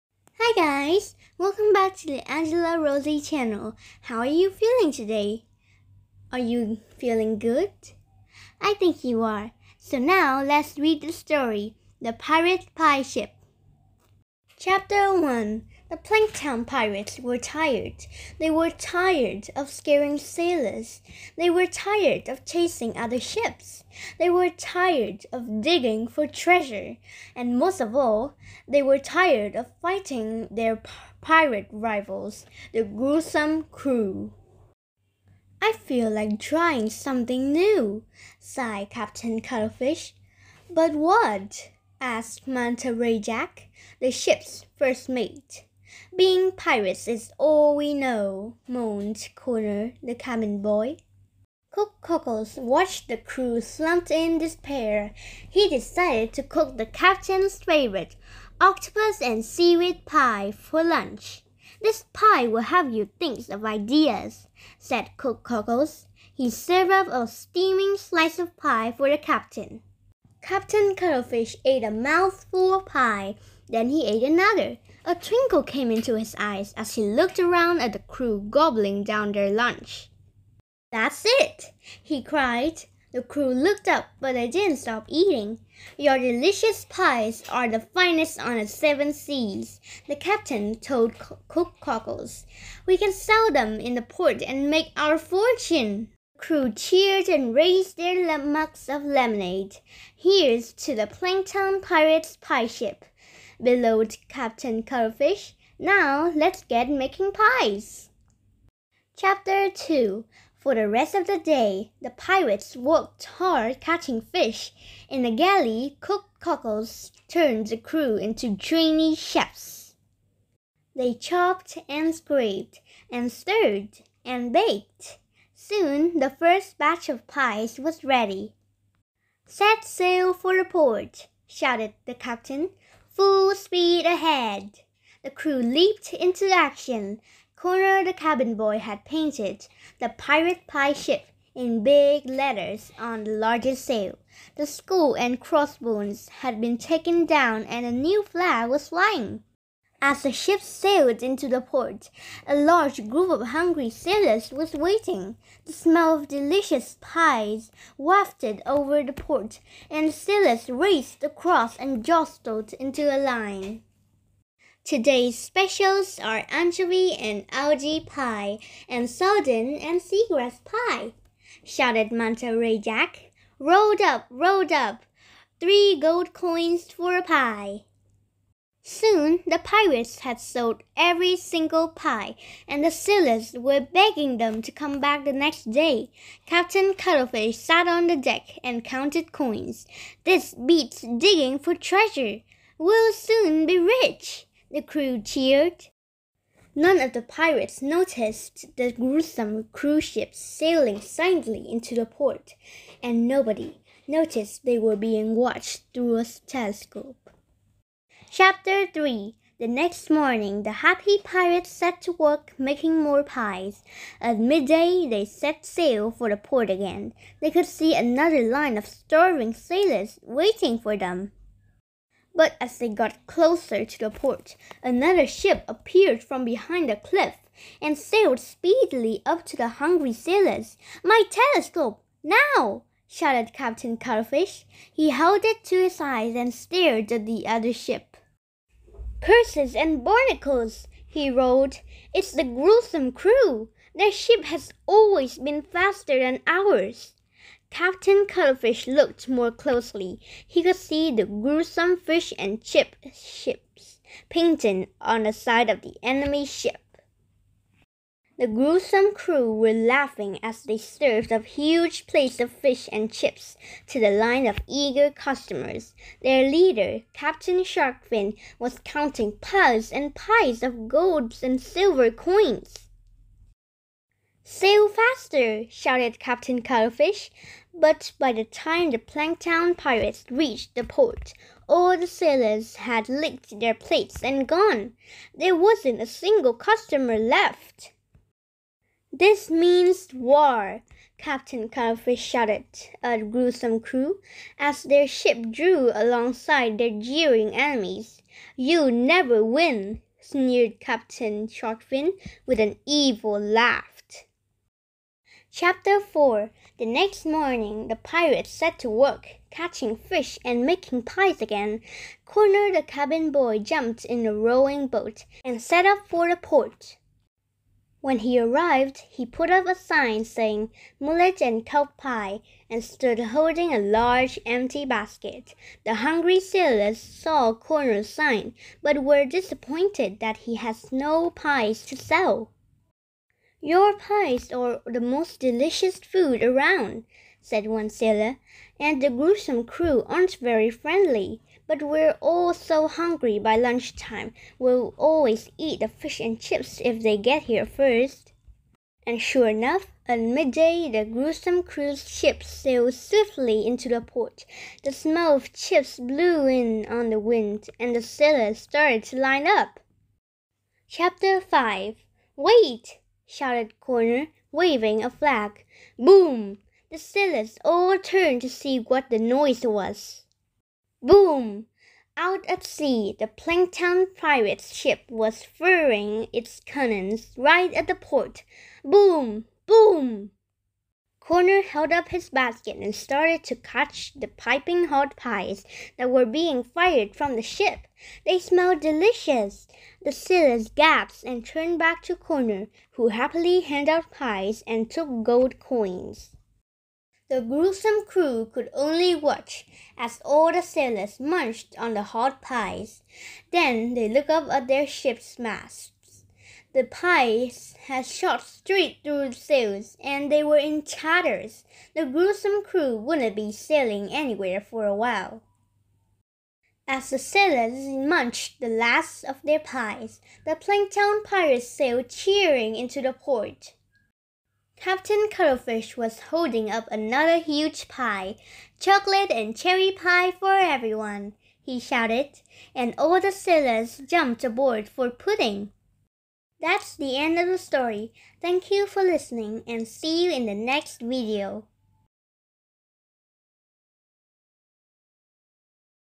The pirate pie ship - Read aloud - Story online
Do you like reading about pirates? This is an interesting story for you - a story about funny pirates. Let's listen to the story to see how funny they are